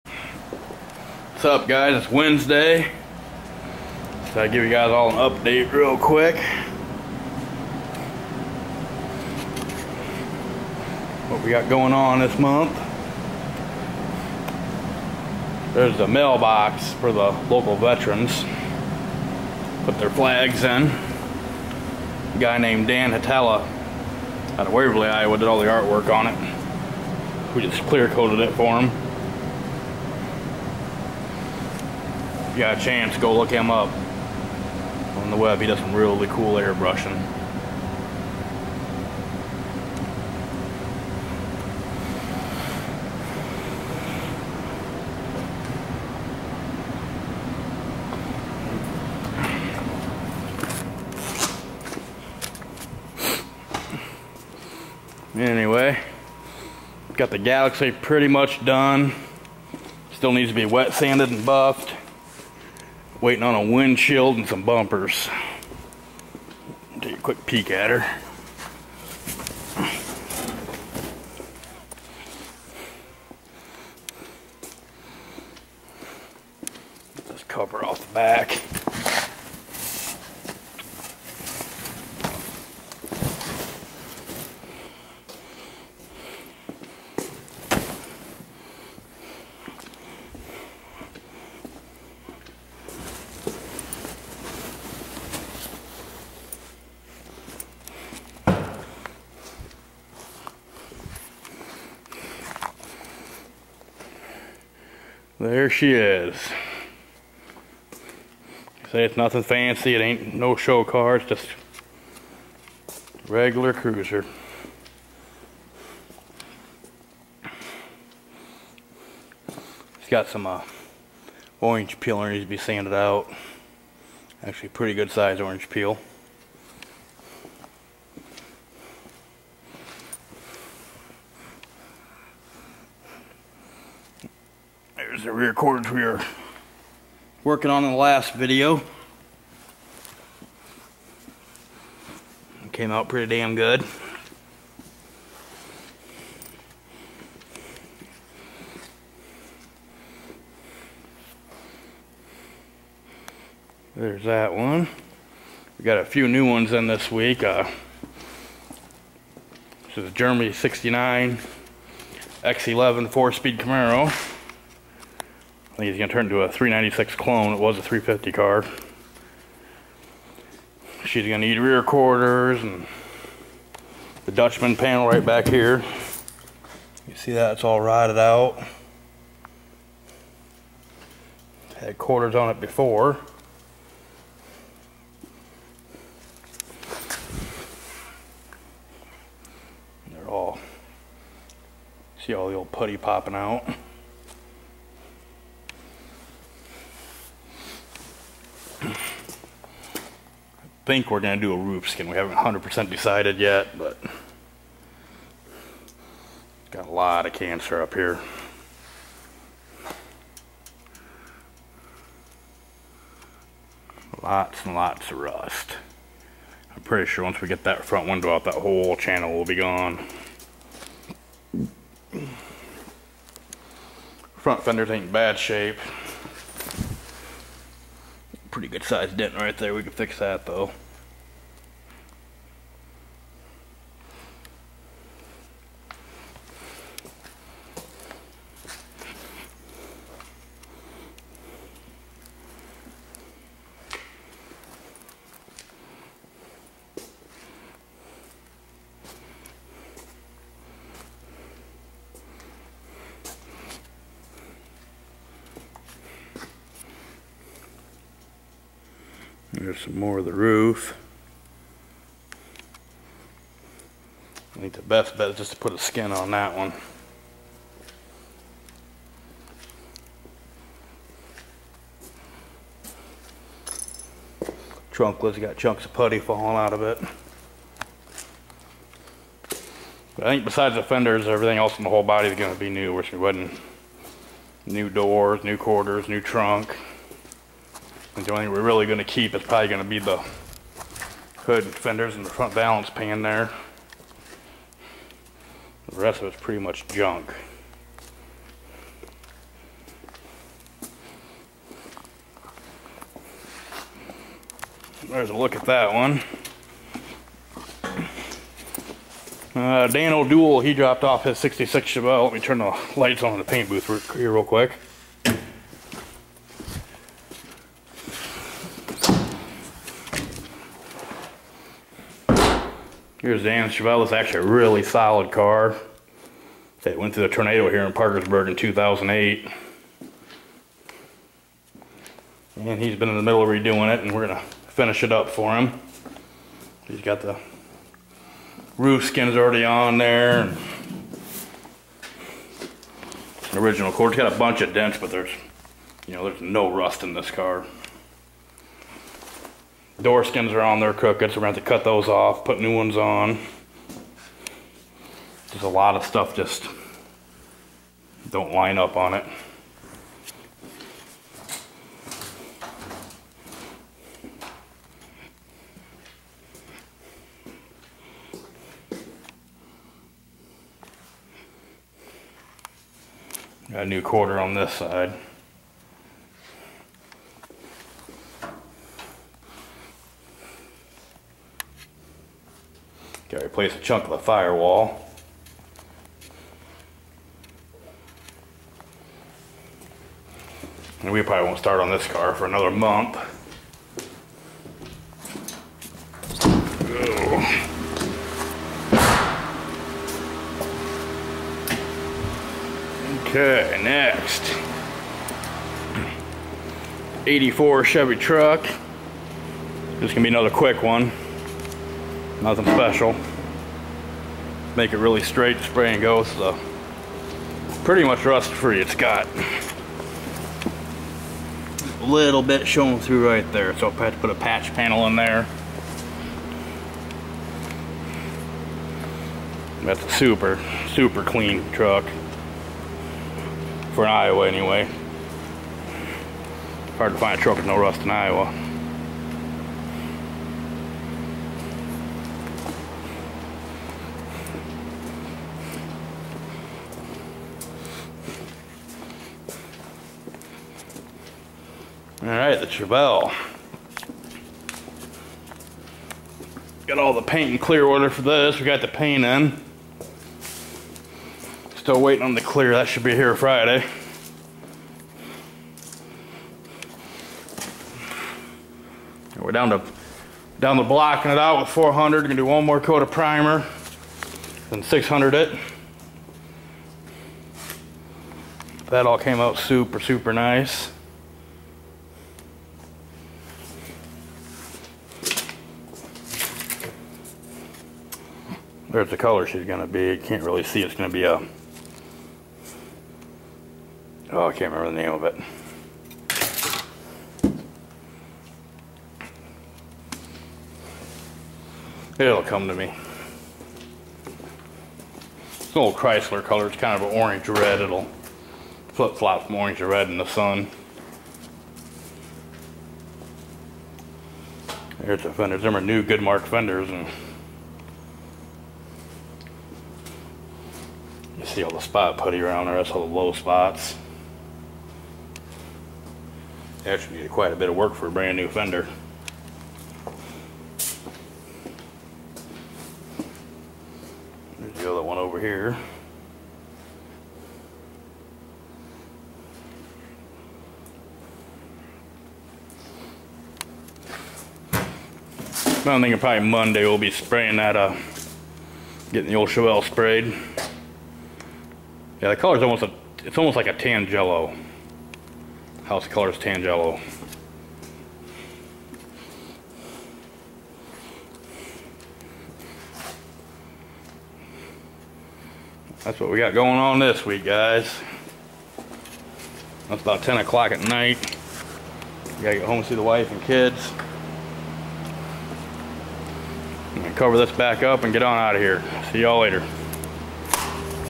shop update 1966 chevelle 1964 galaxie 1984 chevy c10 1970 chevelle 1969 camaro x11
quick walk around the shop a lot of projects going on its crunch time.